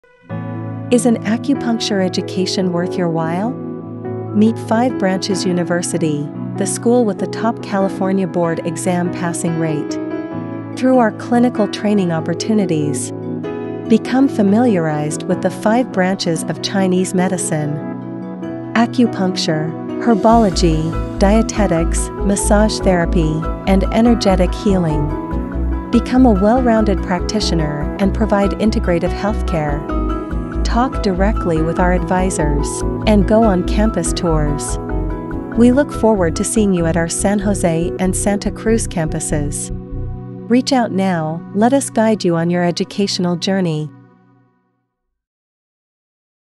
Learn from One of the Top TCM Schools
Join us on Thursday, March 30th for an online orientation session for our Master's and Doctoral programs in Traditional Chinese Medicine.

Our school, Five Branches University, is known for its clinical training and has one of the highest pass rate for the California Acupuncture Licensing Exam. Our students develop professional skills in acupuncture, herbal medicine, dietary therapy, massage, and energy healing.